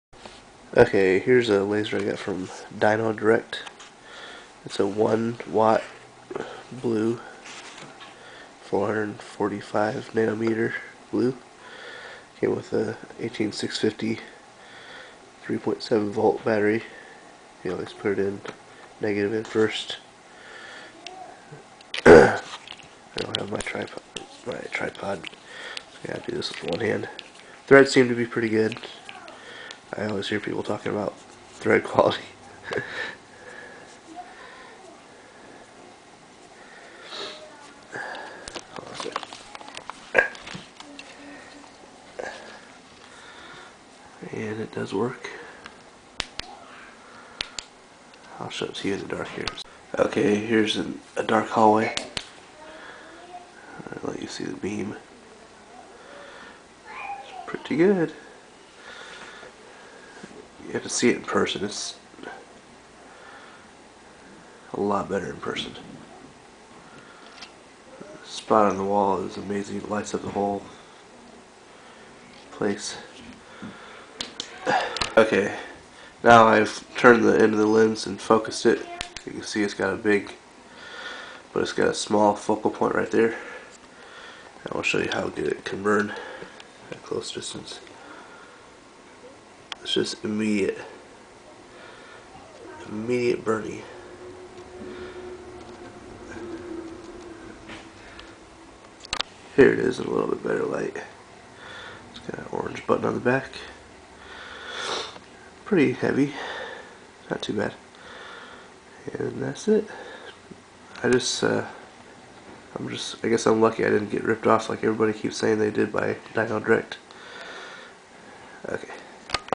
dinodirect 447nm 1000mw laser
This is a very powerful blue laser I just got in the mail from dinodirrect.com
It took 25 days from the time I ordered if to get it in the mail, and after the hassle of not knowing what happened with my order info disappearing from their website, in a little under two weeks I received a tracking number for EMS and USPS.       The laser and the battery charger both work very well, and I have no complaints. For the price, this is a decent laser.